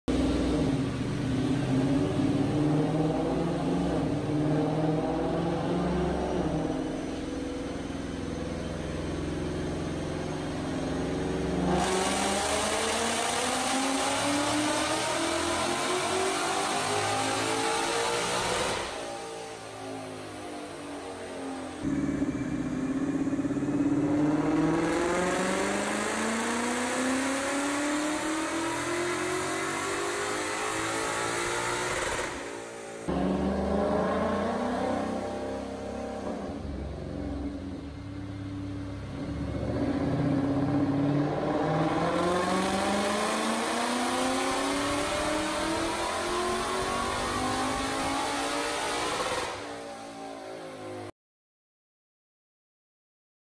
Supercharged RCF with Stage 2 e30
Finally wrapped up this project over here at RR Racing. This RCF came to us straight from the dealership and spent its newborn life getting ready to take on the world. It has:

RR Racing Valvetronic Exhaust
RR Racing PPE Headers
RR Racing USRS
RR Racing Supercharger
Stage II Kit
RR Racing Big Brake Kit

It happily laid down a solid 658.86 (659) WHP & 516 TQ.